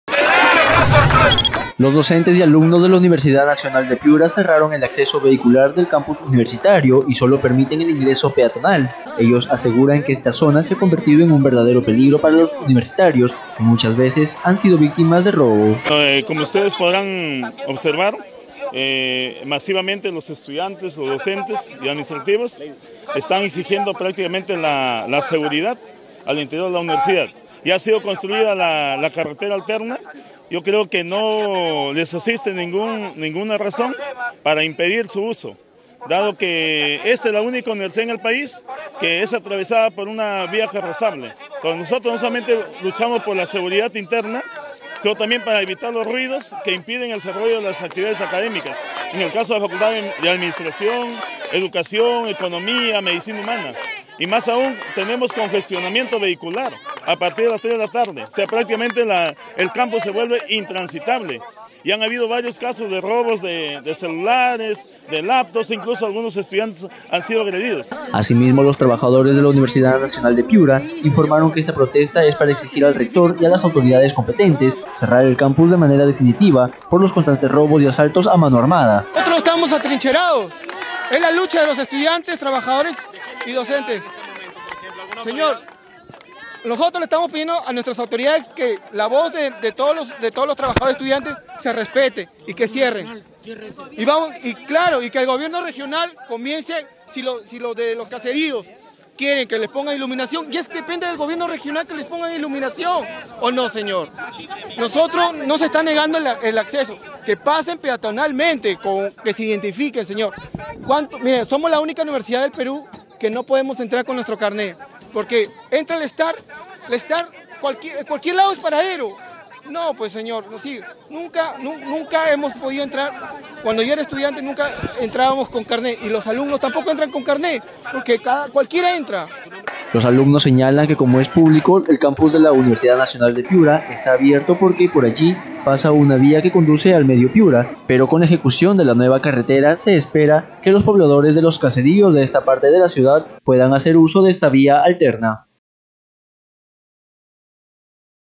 Piura: docentes y alumnos exigen cierre de vía
Docentes y alumnos de la UNP cerraron la principal vía que conecta a la ciudad con el medio Piura, ellos aseguran que esta carretera se ha convertido en un peligro para los universitarios.